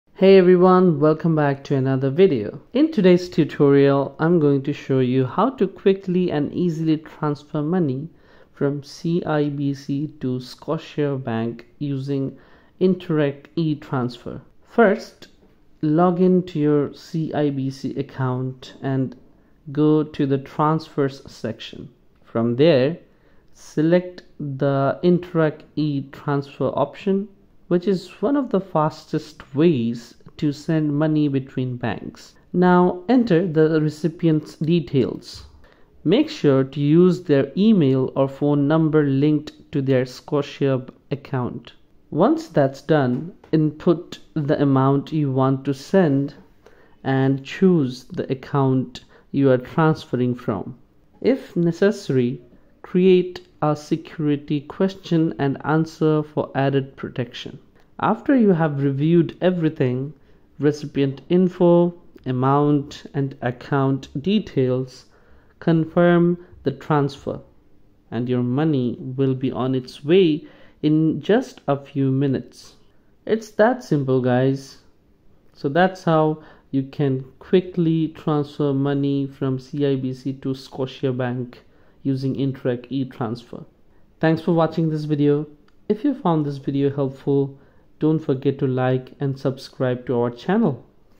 Transfer Money From CIBC To Scotiabank(Easy Step-by-Step)
Transfer Money From CIBC To Scotiabank : Learn how to transfer money from CIBC to Scotiabank in this simple step-by-step guide! We’ll walk you through the process using online banking or the mobile app, making it easy to send funds between accounts securely and quickly. Perfect for CIBC and Scotiabank customers!